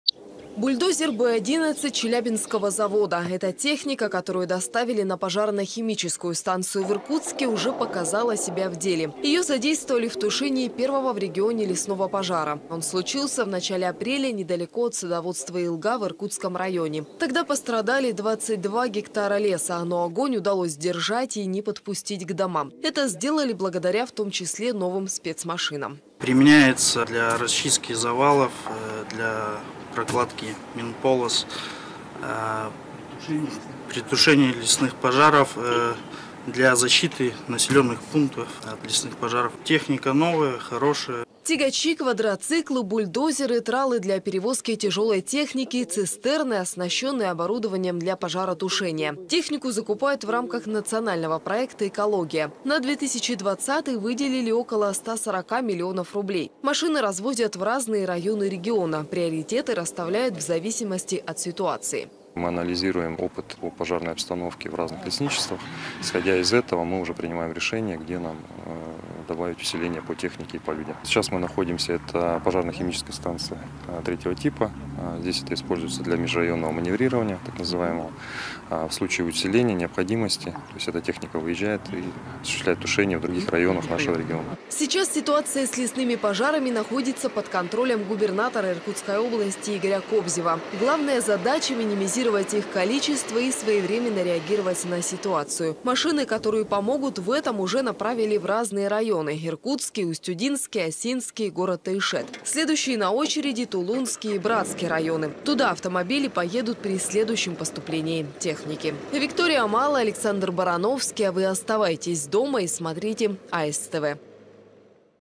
Новая пожарная техника
Семь пожаров обнаружили, шесть ликвидировали. Это статистика возгораний по лесному фонду за прошедшие сутки в Иркутской области. Горело в Братском и Зиминском районах, Боханском, Нукутском и Тайшетском муниципальных образованиях. В тушении задействовали 20 единиц техники. Именно таким количеством машин совсем недавно пополнился региональный парк автомобилей для тушения лесных пожаров.